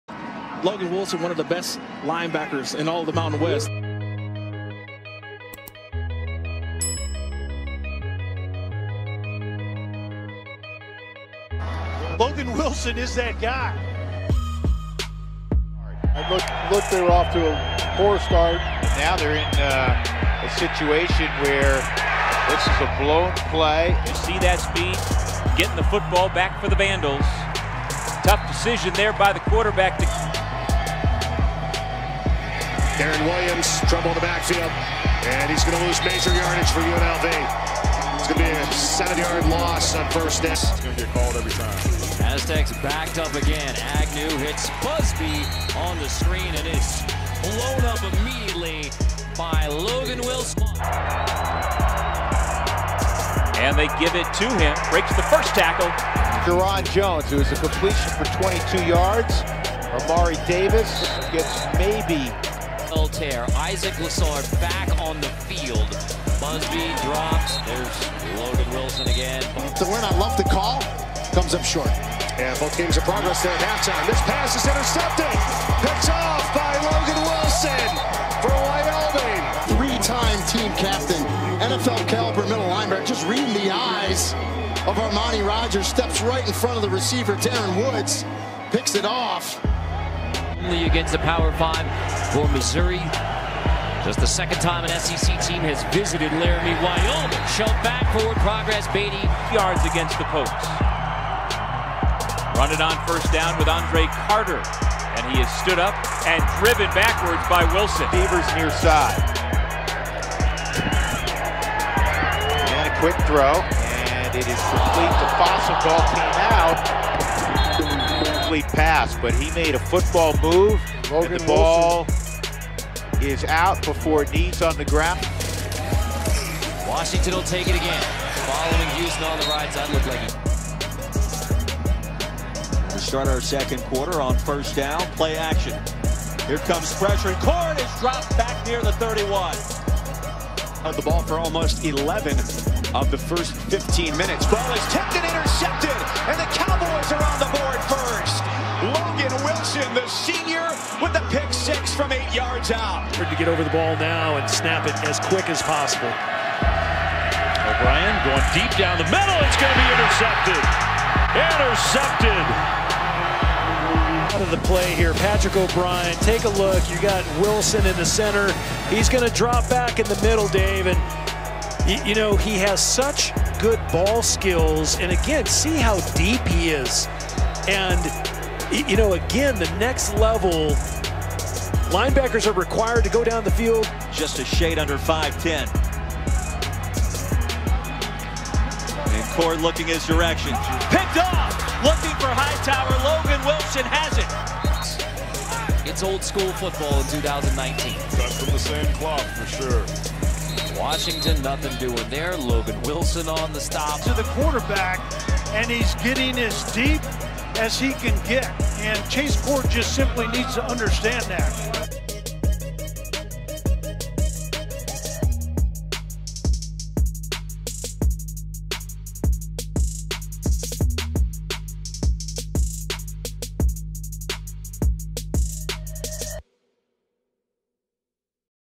Logan Wilson || Wyoming Cowboys Linebacker || 2019 Highlights
All-American Logan Wilson Senior season highlights. 105 Total Tackles, 8.5 Tackles for Loss, 1 Sack, 1 Forced Fumble, 4 Interceptions. 1st Team Mountain West.